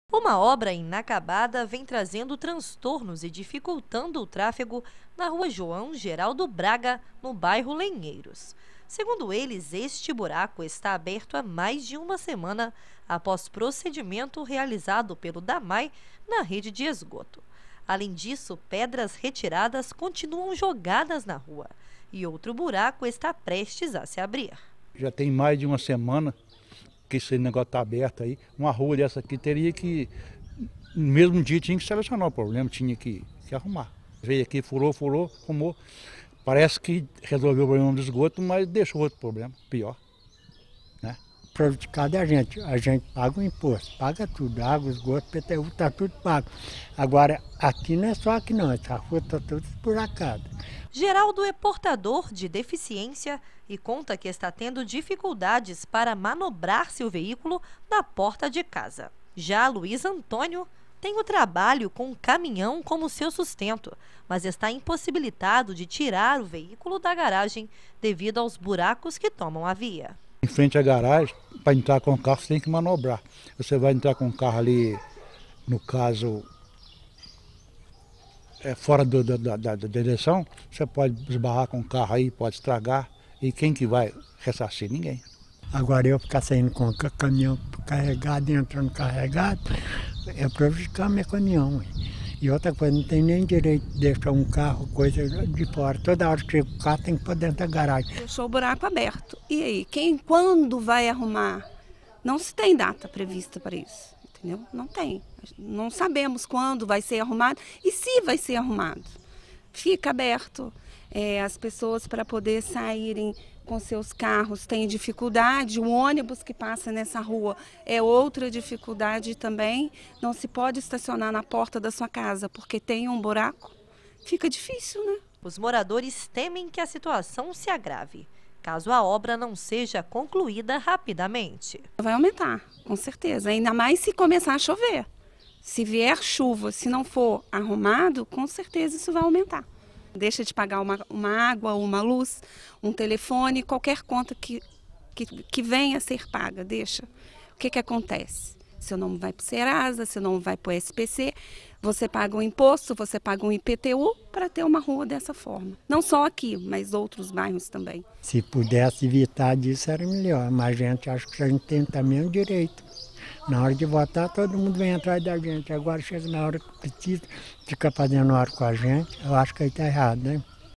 Moradores do residencial Lenheiros, em São João del-Rei, reclamam de obra inacabada. 23/08/16
Moradores da rua João Geraldo Braga, no residencial Lenheiros, reclamam da precariedade do local. Segundo eles, o DAMAE iniciou uma obra para consertar a rede de esgoto que apresentava problemas, no entanto, o trabalho não foi concluído e o tráfego está sendo  prejudicado por buracos.